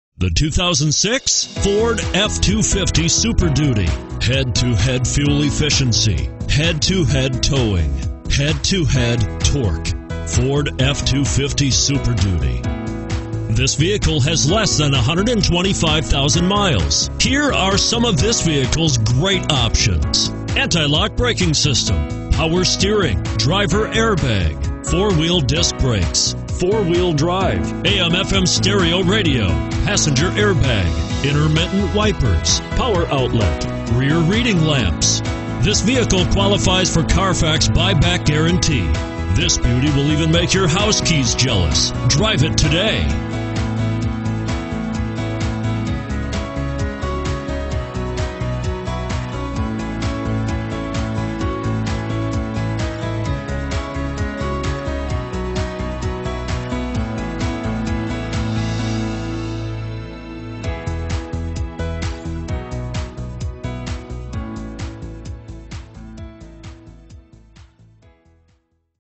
2006 Ford F-250 Super Duty Garland TX E8259A1
2006 Ford F-250 Super Duty Crew Cab 156 XLT 4WD

Prestige Garland
3601 S. Shiloh Road

Don't miss this 2006 Ford F-250 Super Duty. It's equipped with Automatic transmission and features a Black exterior. With 124699 miles, You'll want to take this car home. Make a great choice today.